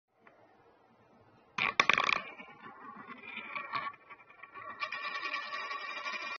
Try this one. Spin a quarter. And try keeping the coin in front of you.
Non-Edited Video Version